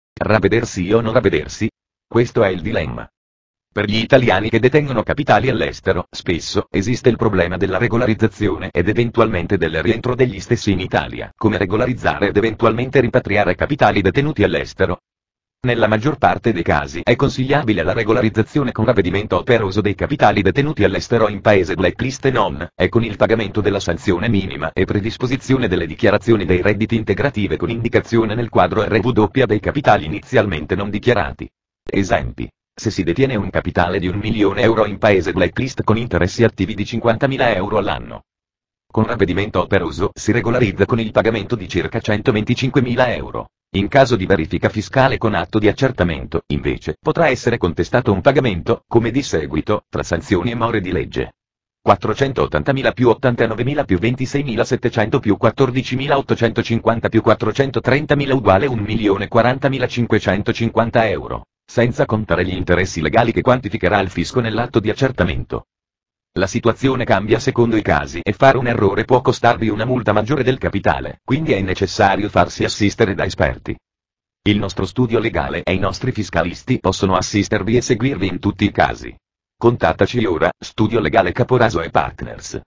Capitali all'Estero: Ravvedersi o Non Ravvedersi?
Scopri se conviene regolarizzare i capitali detenuti all’estero. Analizziamo due scenari: con ravvedimento operoso o con verifica fiscale e atto di accertamento. Quale opzione è più vantaggiosa?
Esempio:
A) Ravvedimento operoso su un capitale di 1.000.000 EUR: paghi circa 125.000 EUR.
B) In caso di accertamento fiscale, potresti pagare oltre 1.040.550 EUR tra sanzioni e more di legge.
Evita rischi con il supporto del nostro studio legale Caporaso & Partners.